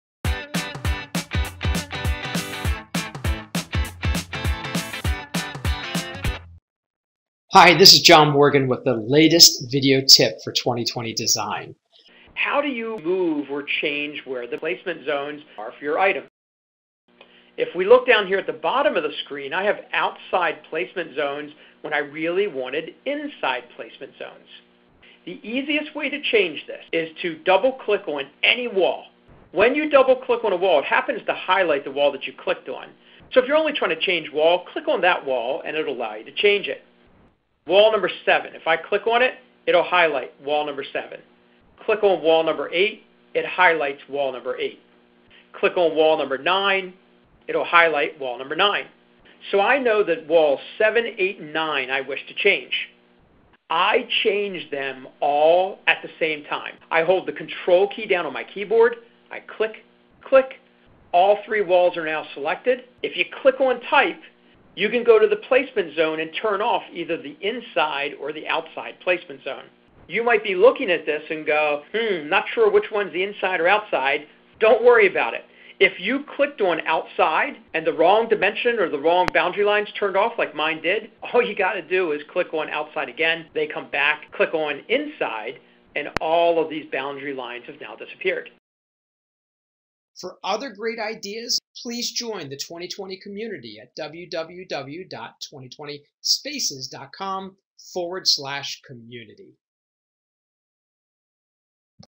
2020 Design Tips: Move Placement Zones
Learn how to adjust your placement zones while working within 2020 Design to maximize your workflow and design your small spaces.